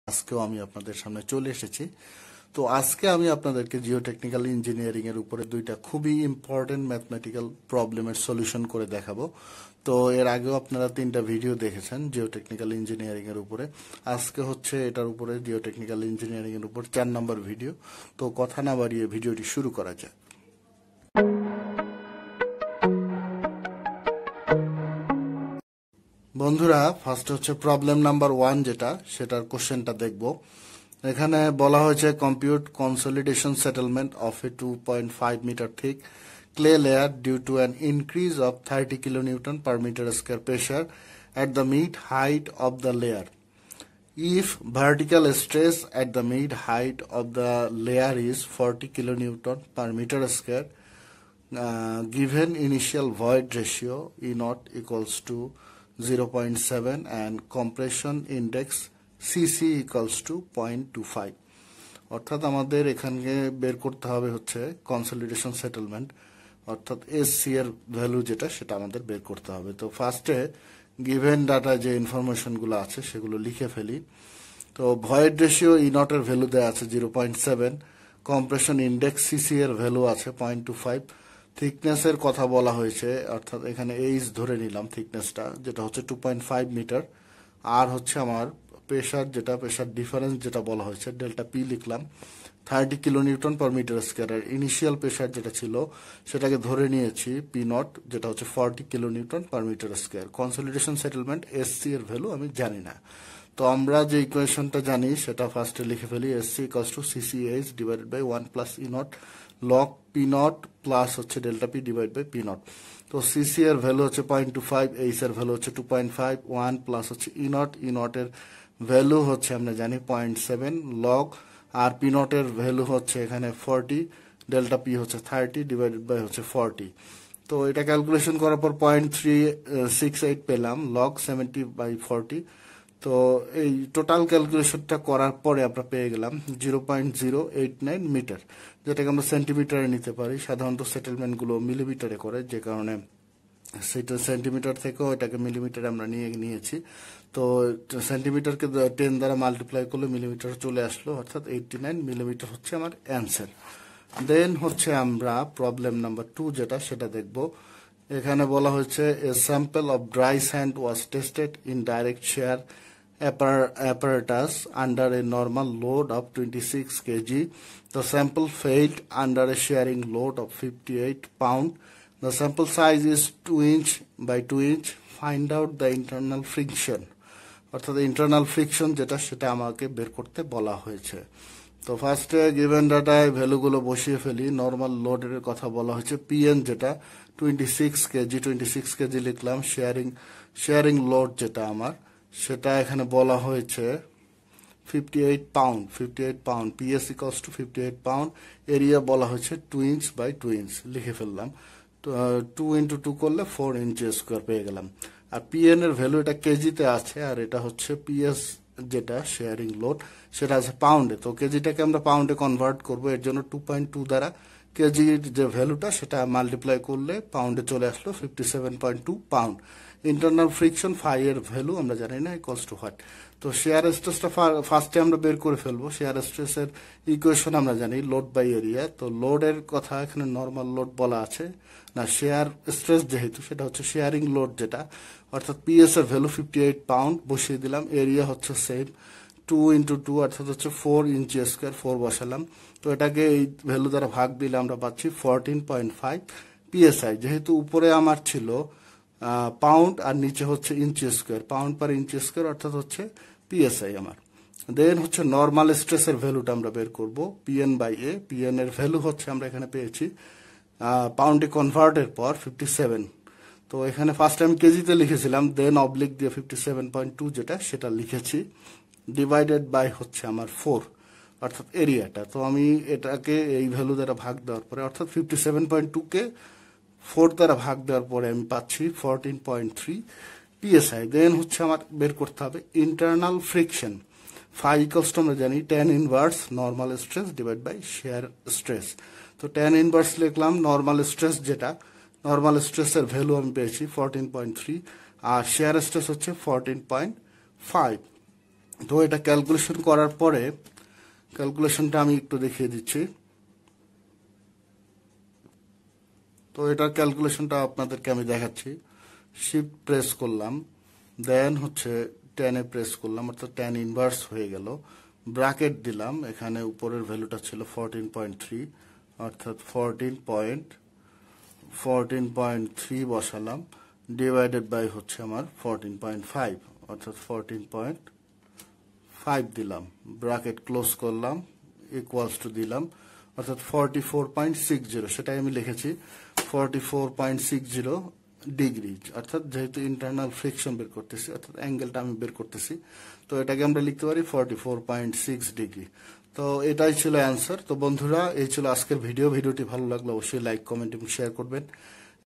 Math Problems: Consolidation Settlement and Internal Friction
Mathematical solutions for the problem of consolidation settlement and Internal friction.